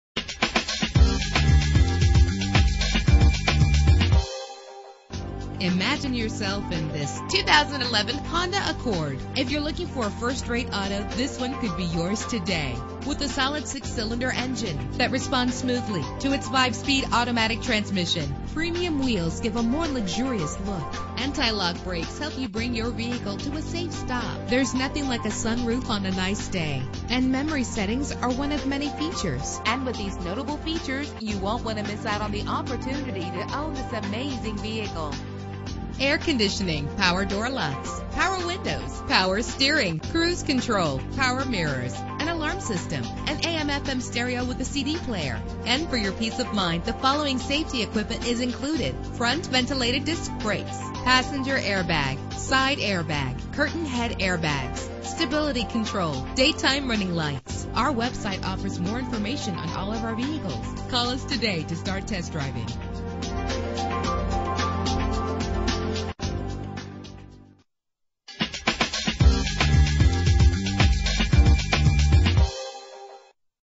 2011 Honda Accord - ASHLAND VA
Year: 2011
Make: Honda
Model: Accord
Trim: 3.5 EX-L w/ Navigation
Engine: 3.5 liter 6 cylinder 24 valve
Transmission: 5-Speed Automatic
Color: White Diamond Pearl
Mileage:
Address:
418 S WASHINGTON HWY